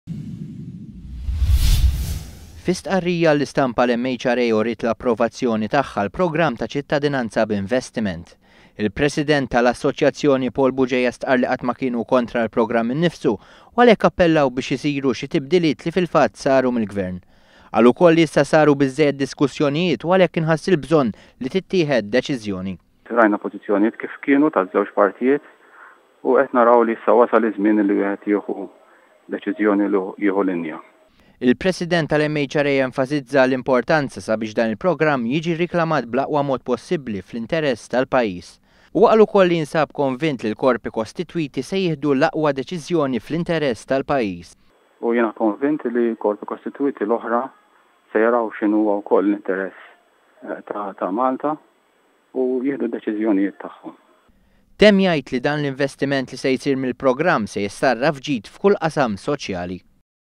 L-MHRA tapprova l-programm ta' ċittadinanza b'investiment.
L-assoċjazzjoni tal-lukandi u ristoranti approvaw il-programm ta' ċittadinanza b'investiment mfassal mill-Gvern. L-MHRA qalet li issa wasal iż-żmien li nimxu l-quddiem biex jidħol fis-seħħ il-programm ta' ċittadinanza b'investiment bl-għan li jkattar l-ekonomija.